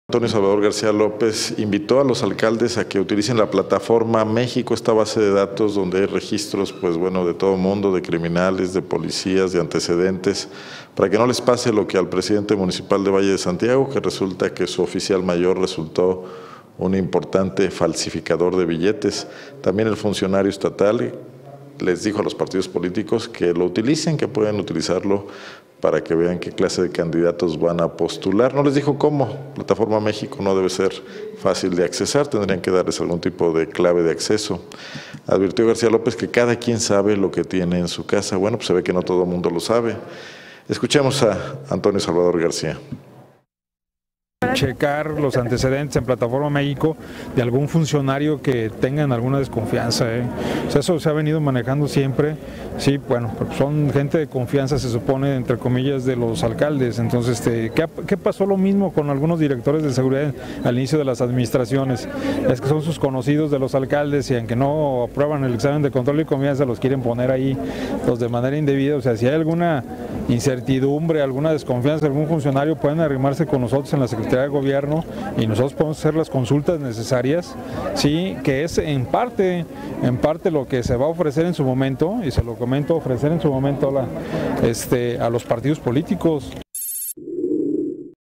Ofrece Secretaria de Gobierno a municipios acceso a Plataforma México
El Secretario de Gobierno Antonio Salvador García López, invitó a los 46 alcaldes del estado a que utilicen la Plataforma México a la cual su entidad tiene acceso, en caso de tener dudas de los antecedentes penales que pudieran arrastras sus funcionarios.